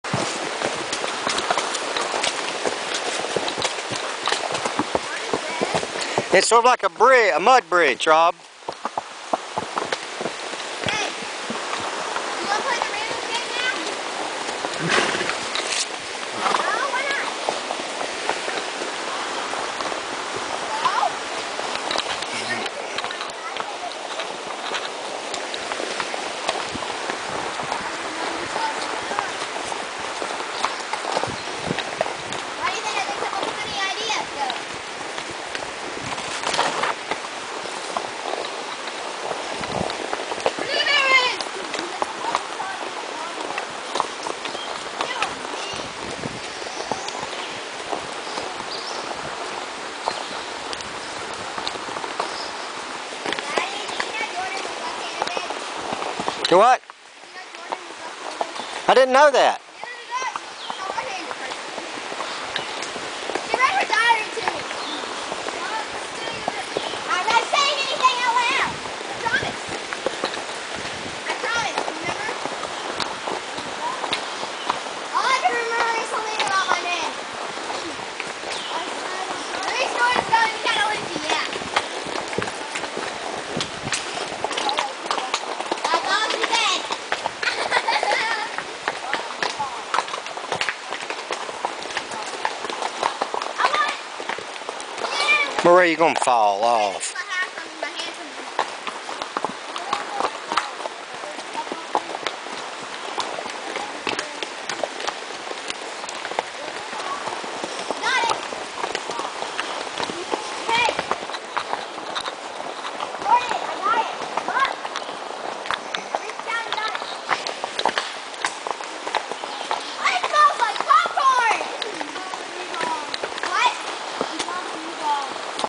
pretty hollow tr.MPG
June 2011 trip to Cataloochee in the Smokey mtns.  This is the ride up Pretty Hollow Trial to the ridge top at Benton McKay trail.